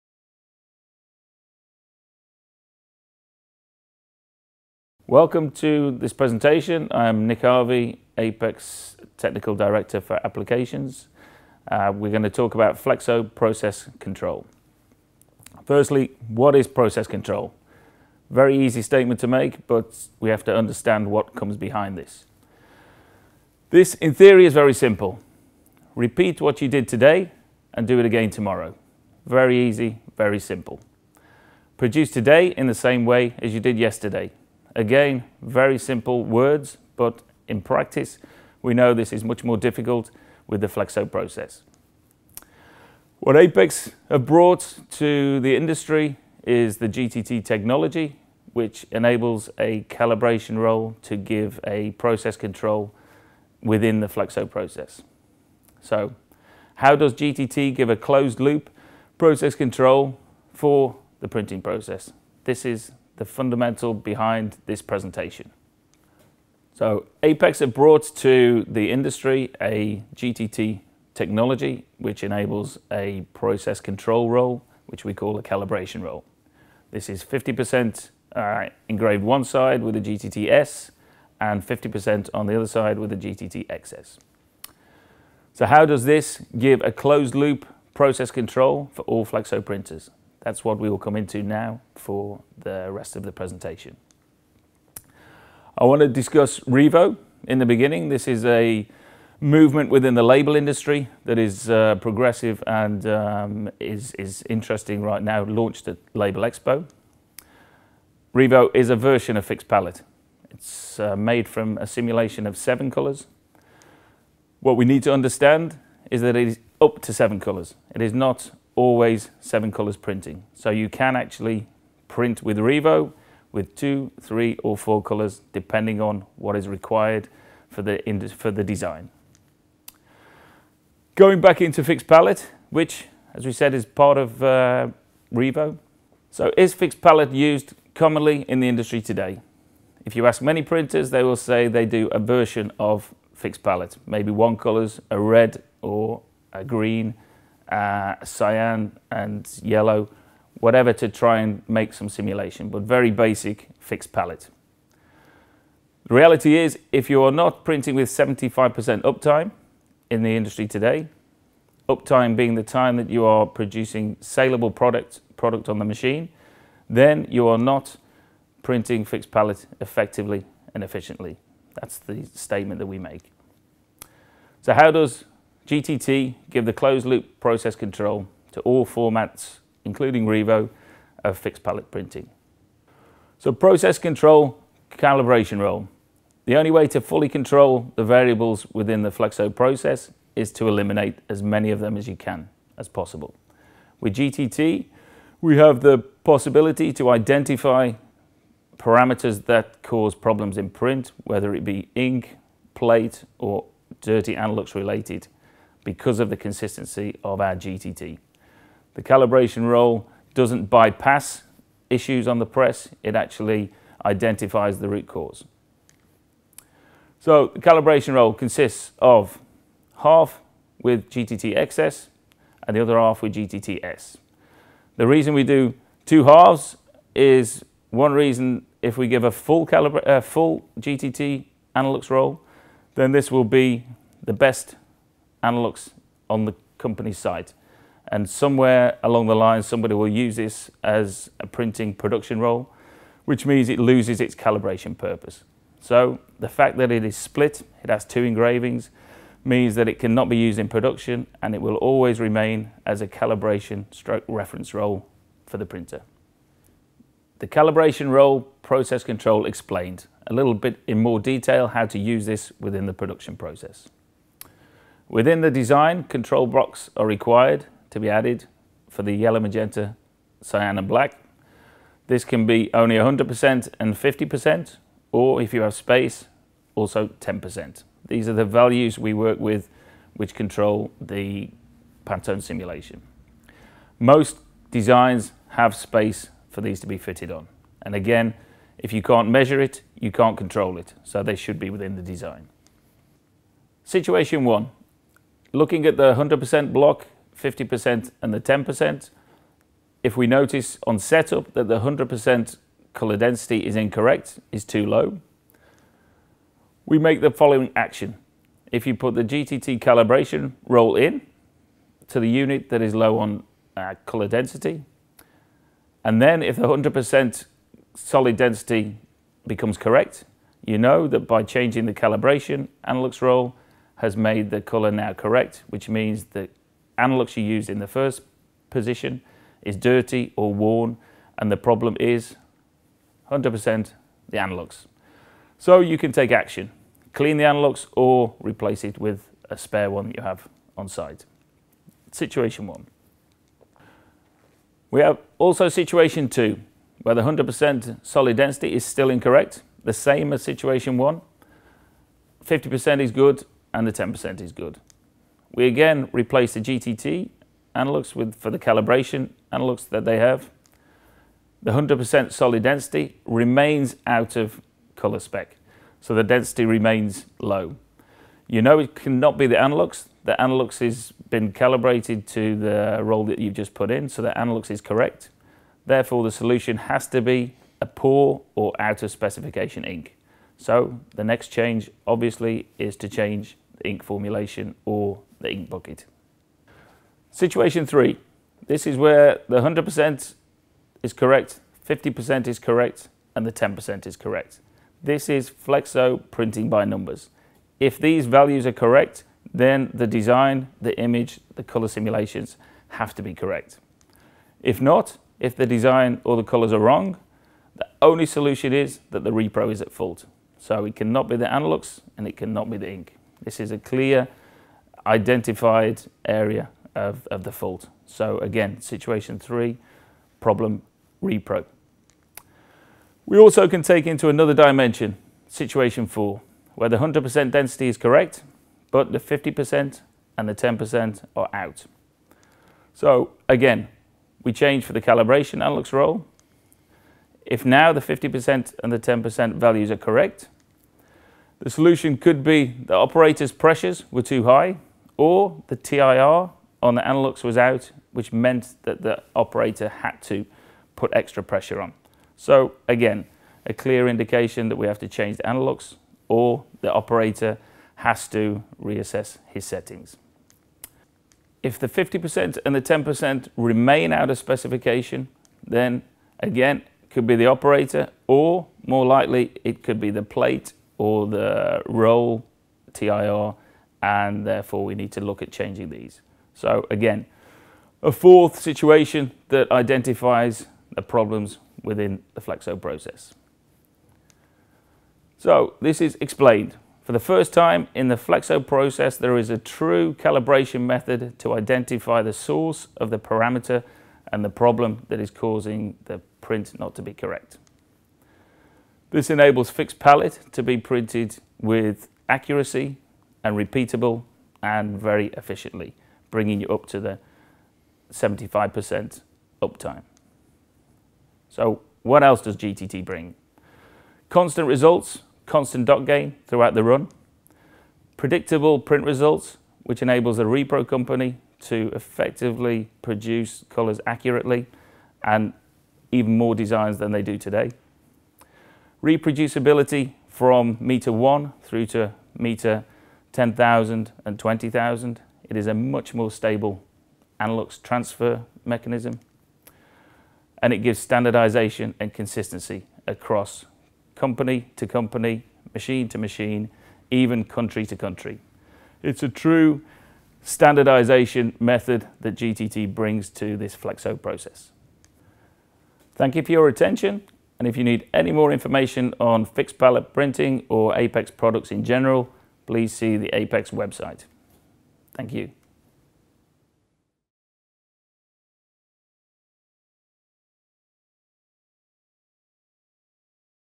Apex PresentationFlexoProcessControl HD
Nick Harvey (Apex International - Technical Director Applications) explains what it takes to obtain Flexo Process Control, necessary for Fixed Palette printing. The theory behind the calibration process is presented as well.